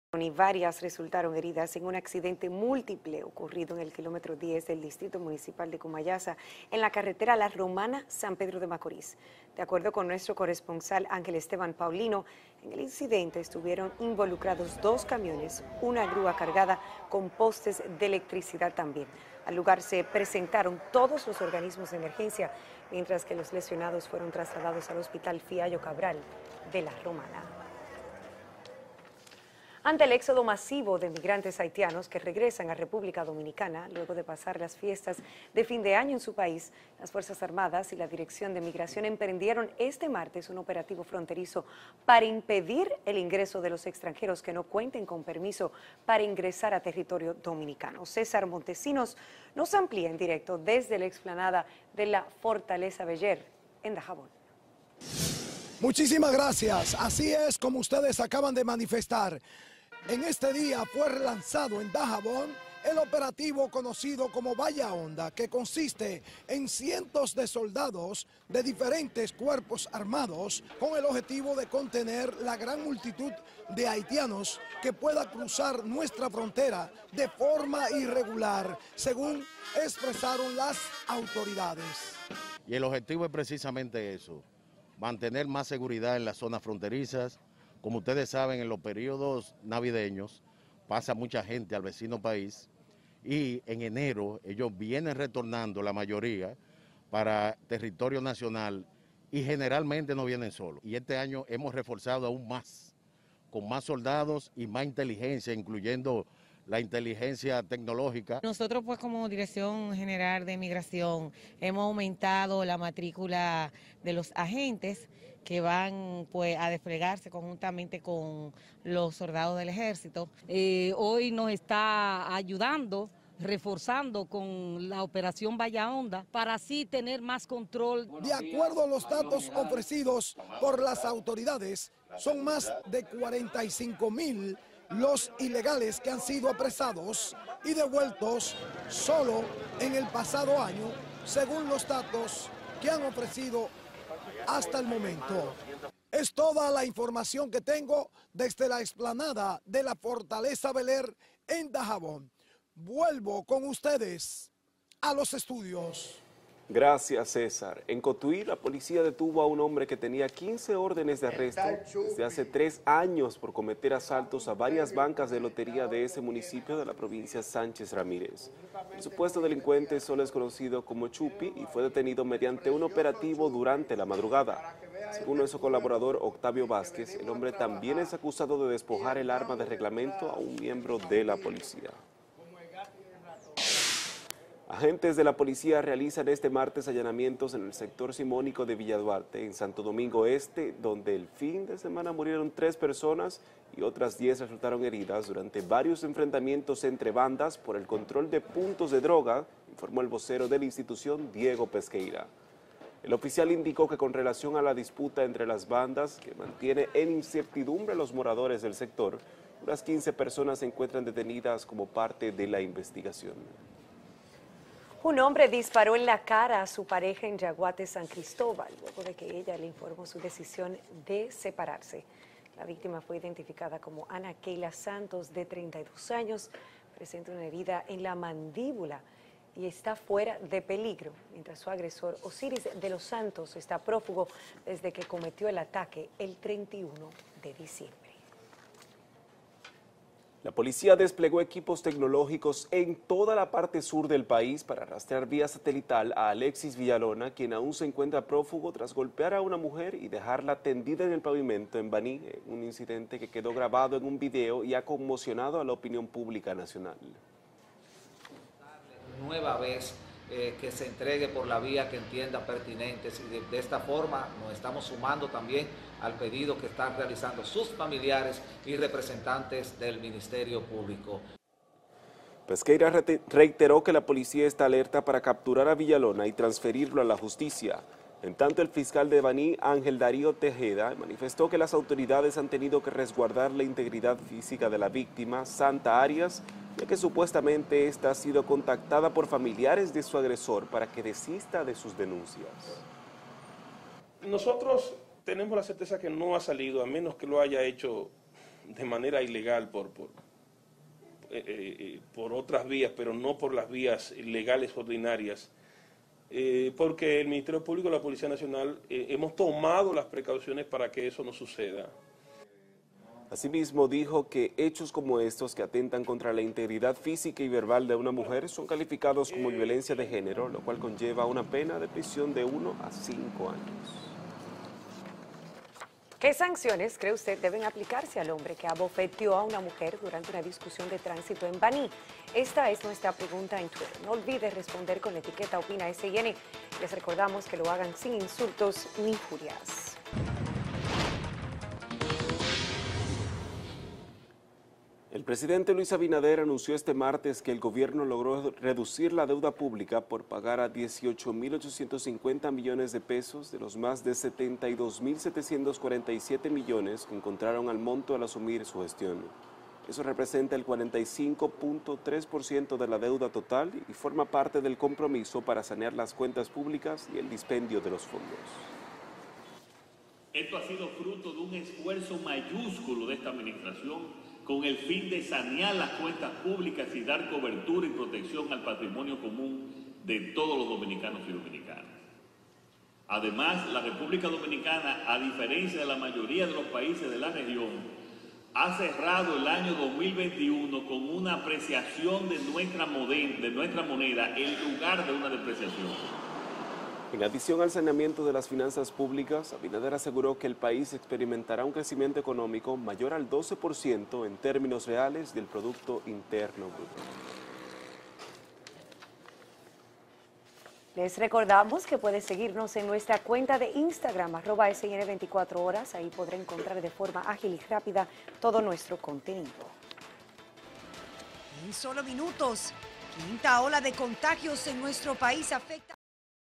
PrimeraEmisión: Lanzan operativo y hombre dispara a pareja
La Policía desplegó equipos tecnológicos en todo la parte Sur del país para rastrear vía satelital a Alexis Villalona, quien aún se encuentra prófugo tras golpear a una mujer y dejarla tendida en el pavimento en Baní en una incidente que quedó grabado en video y ha conmocionado a la opinión pública nacional.